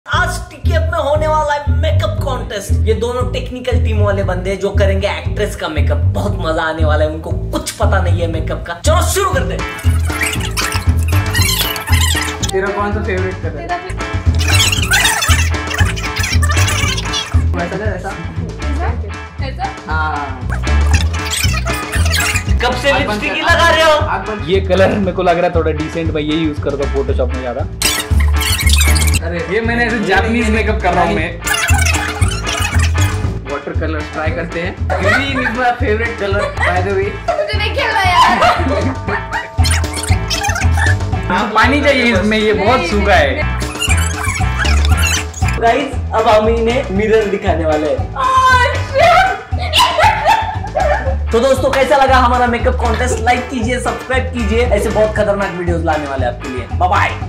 क्या हुआ जब लड़को ने किया लड़कियों का makeup? 😱🤣 TKF Makeup Challenge
Go Check out and get yourself a FAADU T-shirt right away! TKF ke team ne kiya makeup challenge. Janiye kaun jeeta aur kaun haara.